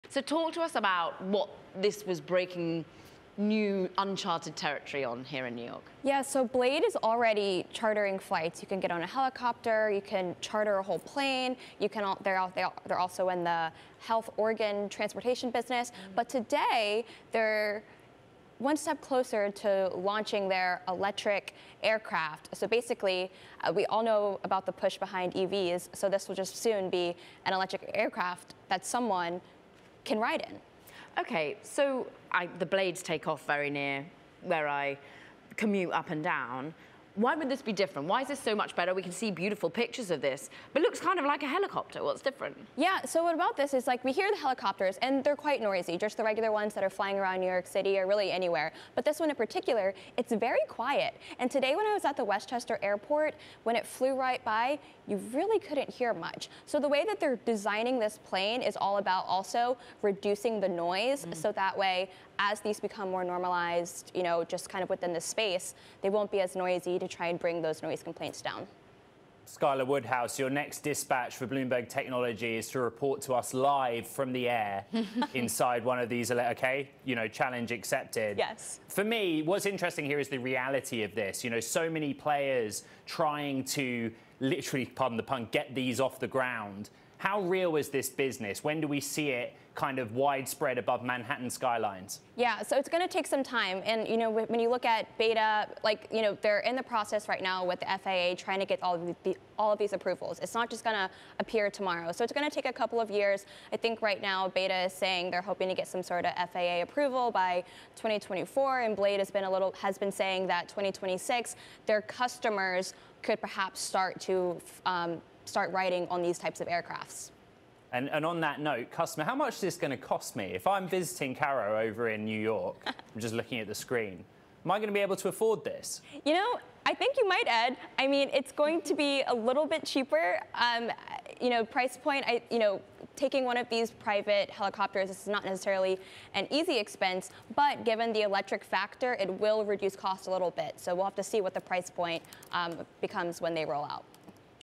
Going Viral: First Electric-Vertical Air Flight in NYC Area
Bloomberg's Skylar Woodhouse joins Caroline and Ed Ludlow to discuss Blade Air Mobility and Beta Technologies' successful completion of a historic electric vertical take-off and landing test flight in White Plains, New York   the first of its kind in the area.